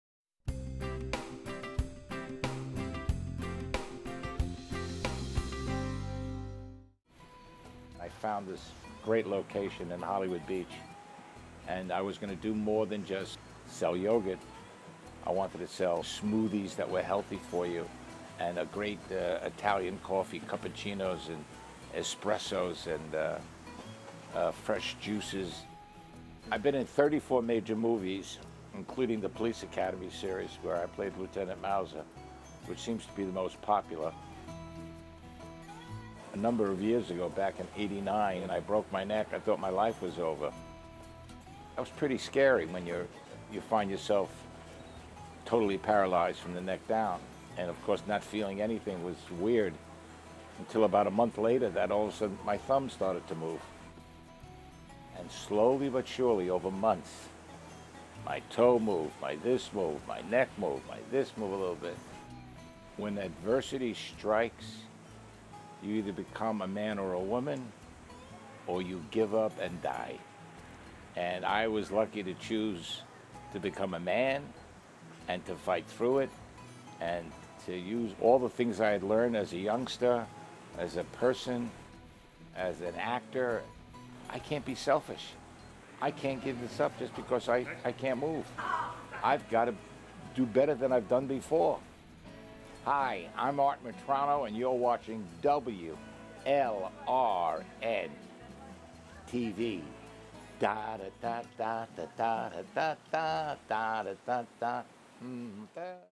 Miracle Man, Art Metrano - Slice of Life
Take a look at a yogurt store in Hollywood Beach, and learn the surprising and intrepid history of it's owner!